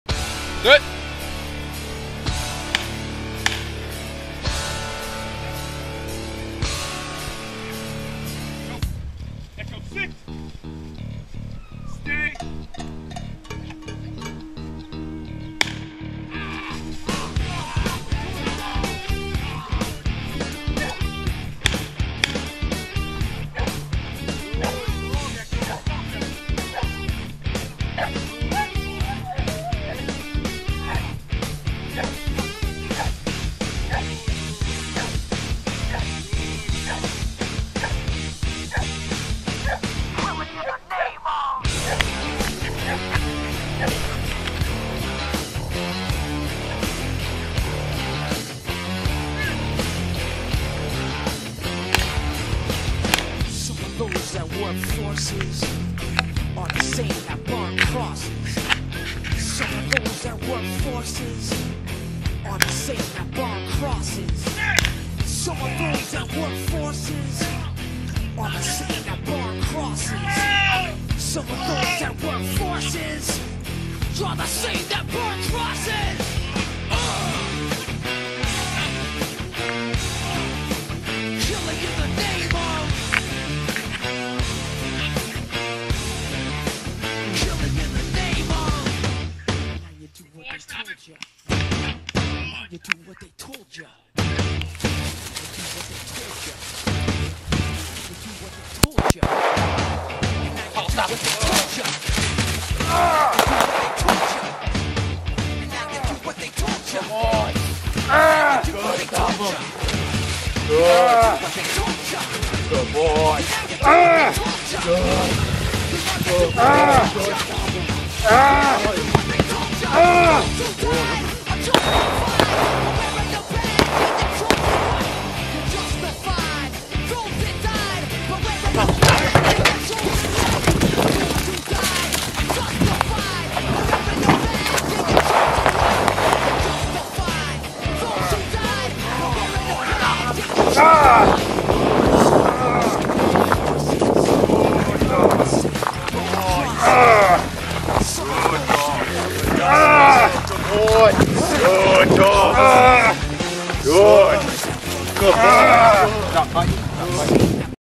Vehicle Work/Gunfire
Implementing weapons and vehicles into working dog training.